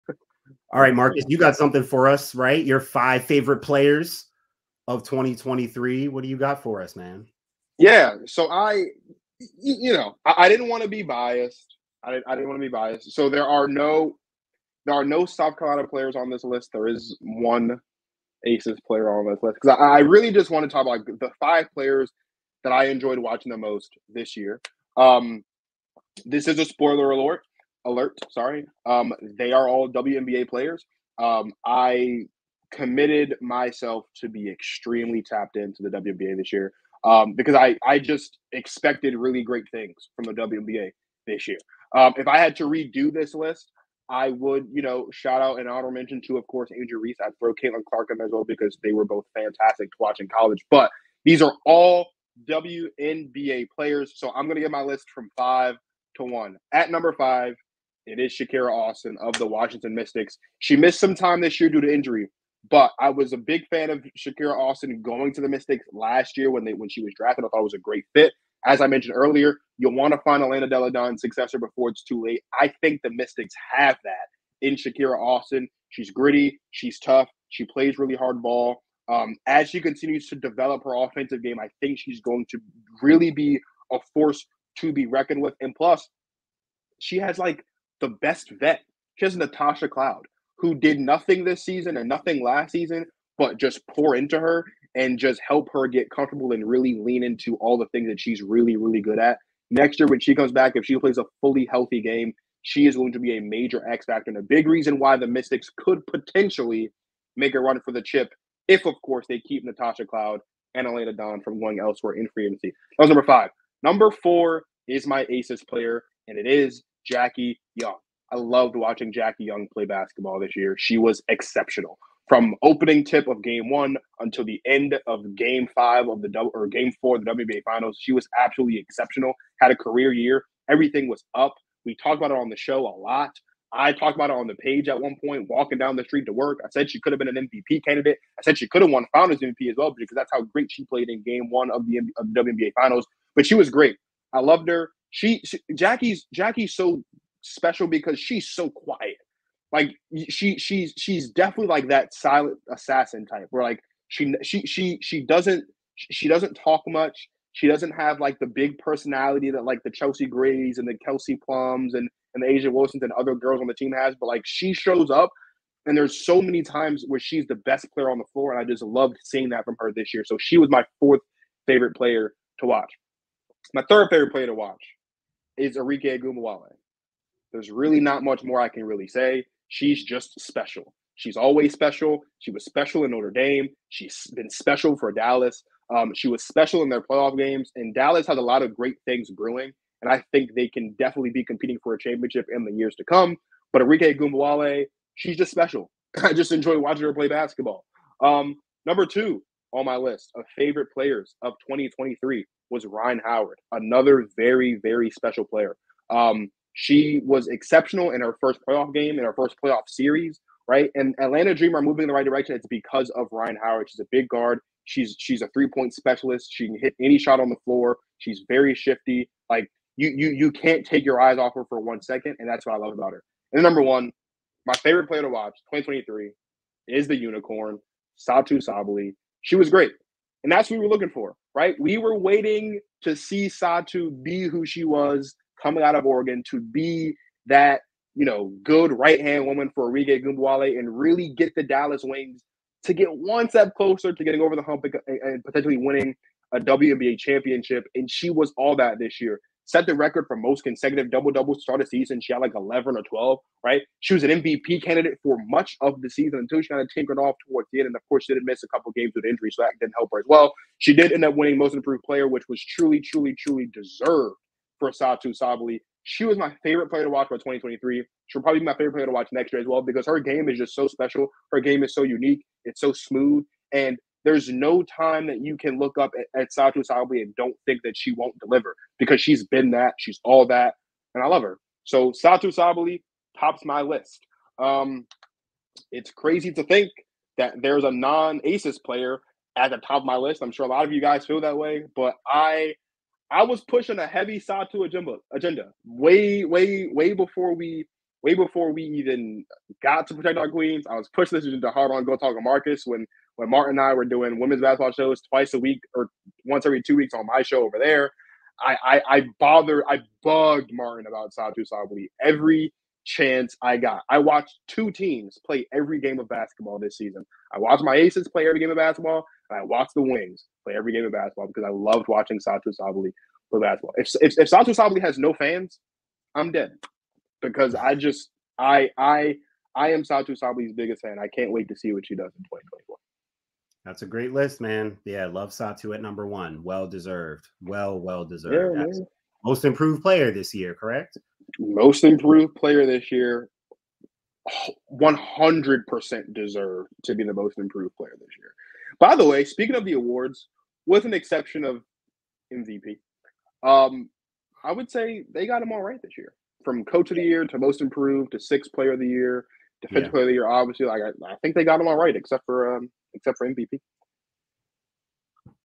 Top 5 WNBA Players To Watch In 2023 | Protect Our Queens
Dwayne Marcus gives his top 5 players in 2023.